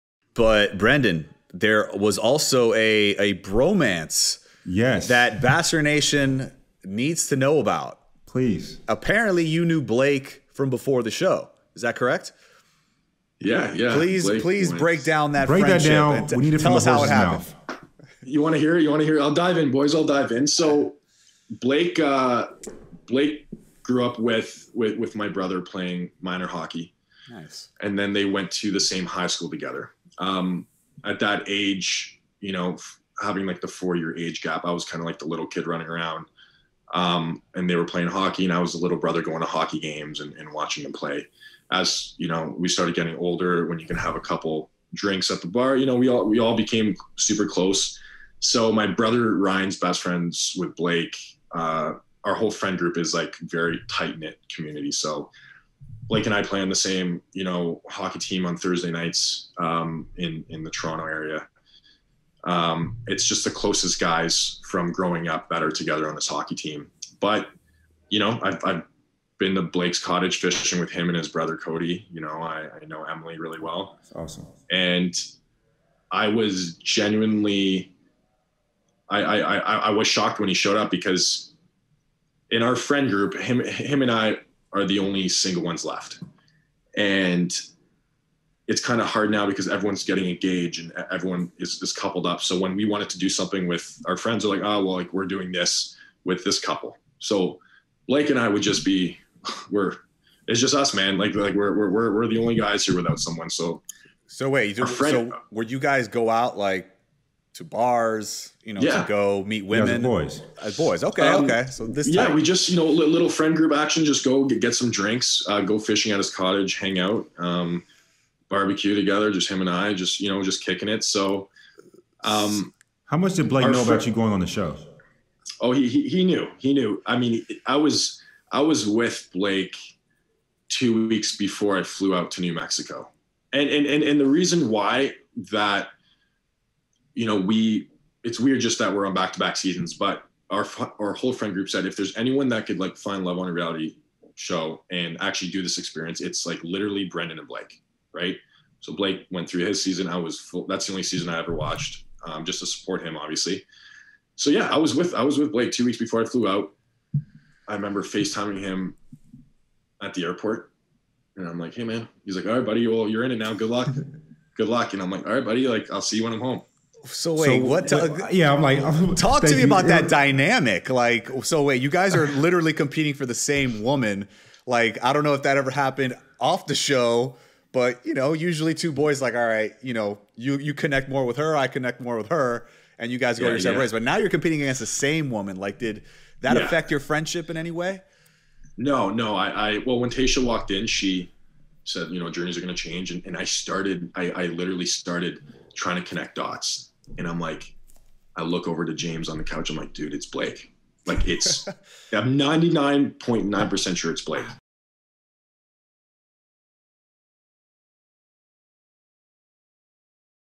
‘The Bachelorette’s’ Brendan Scanzano Clears Up His Friendship with Blake Moynes
Brendan Scanzano chats with co-hosts Mike Johnson and Bryan Abasolo about his experience on “The Bachelorette” Season 17. Brendan explains how he knew fellow contestant Blake Moynes from back home in Toronto before going on the show. Brendan also shares his thoughts on Blake crashing the competition for Katie Thurston’s final rose.